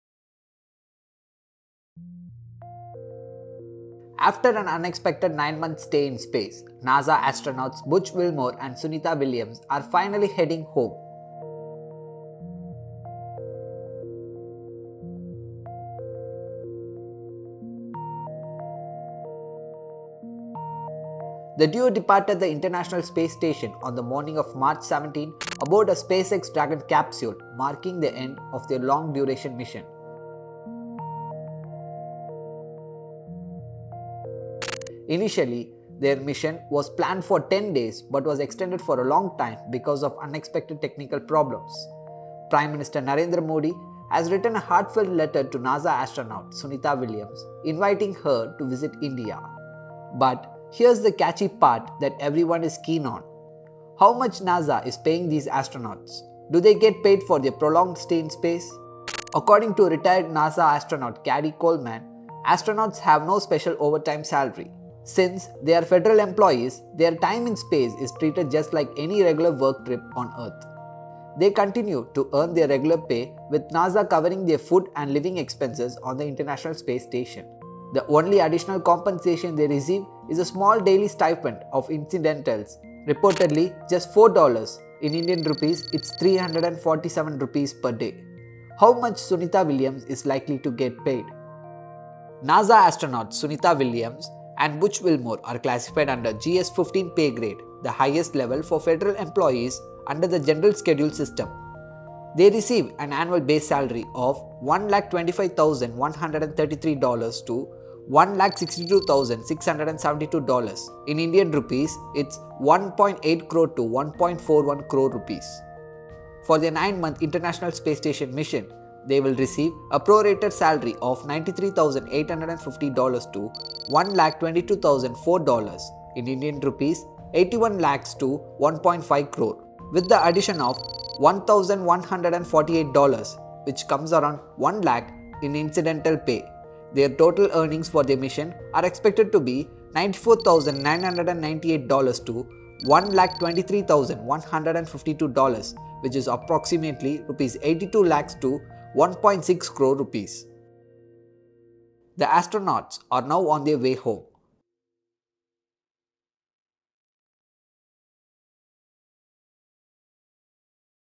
How Much Will Sunita Williams Get Paid for Her 9-Month-Long Stay In Space? | Rs 347 a day?
After an unexpected nine-month stay in space, NASA astronauts Butch Wilmore and Sunita Williams are finally heading home. But here’s the big question—how much is Sunita Williams likely to get paid? Do astronauts receive extra pay for prolonged space missions? Does NASA compensate them for extended stays? Let’s break it all down in this video.

Visuals - NASA